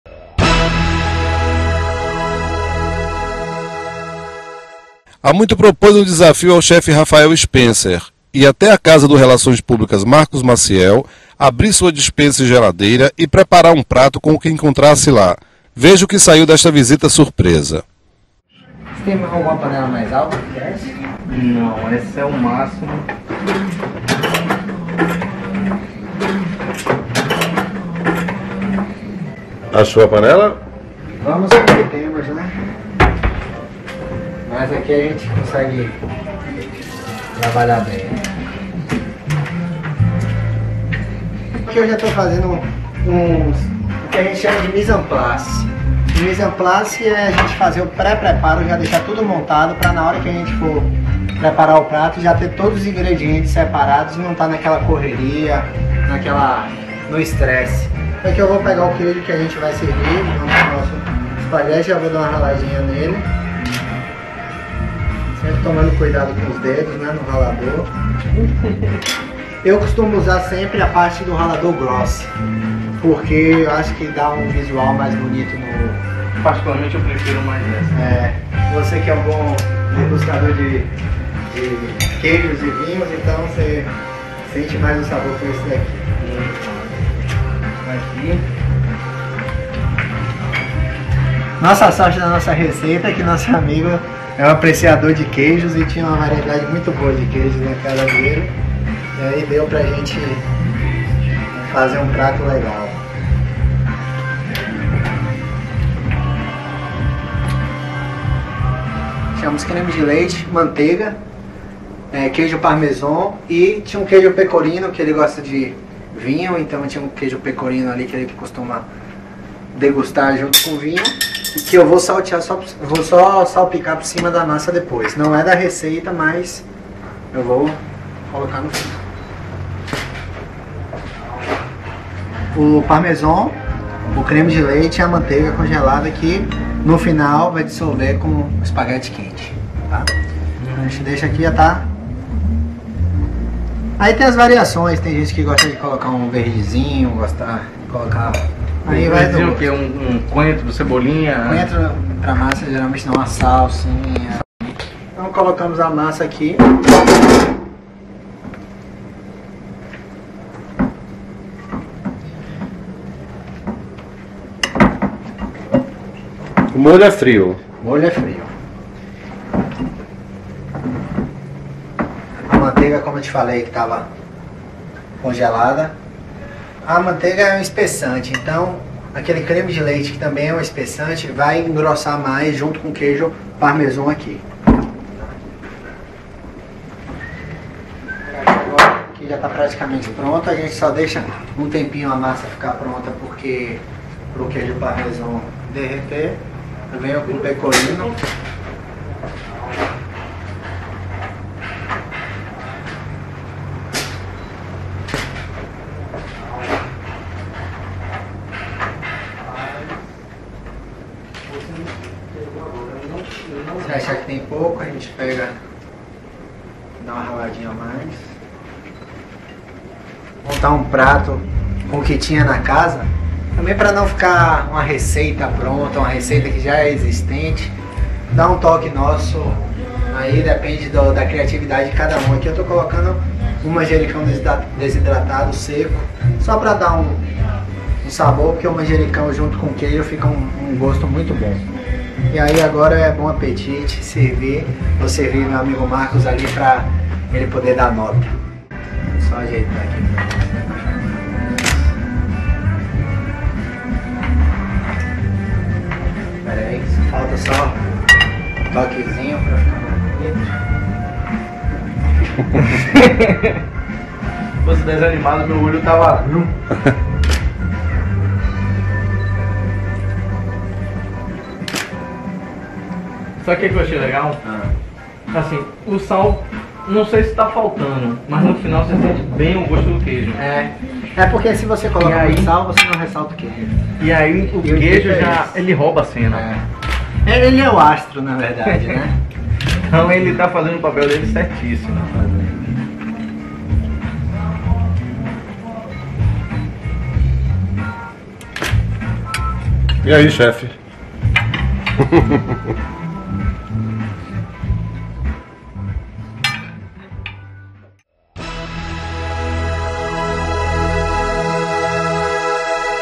MUITO - Spaghetti al triplo burro
O chef Rafael Spencer chegou de surpresa à casa do relações-públicas Marco Maciel. Com o que encontrou na geladeira e na despensa, improvisou um spaghetti  al triplo burro.